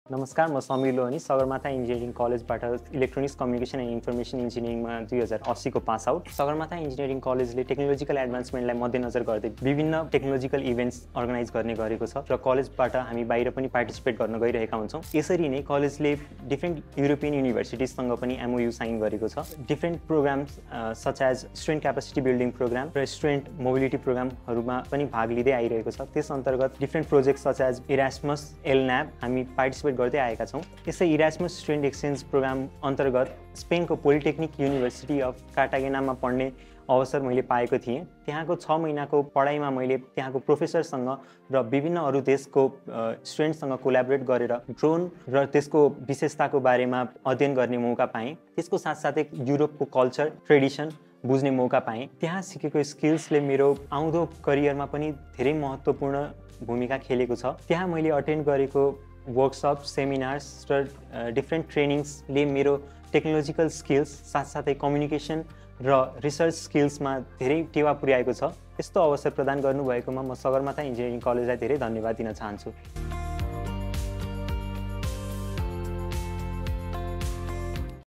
Alumni Spotlight: Sameer Lohani, alumnus of Sagarmatha Engineering College
🎓 Alumni Spotlight: Sameer Lohani, a proud alumnus of Sagarmatha Engineering College, reflects on his incredible college journey! 🌟

From academic growth to unforgettable memories, Sameer shares how his experience at Sagarmatha shaped him into who he is today. Plus, during his time at college, he had the amazing opportunity to participate in a 6-month student exchange program at the University of Cartagena in Spain 🇪🇸.

The insights he gained abroad not only broadened his horizons but also equipped him with valuable knowledge that he continues to implement in his professional life today. 🌍🚀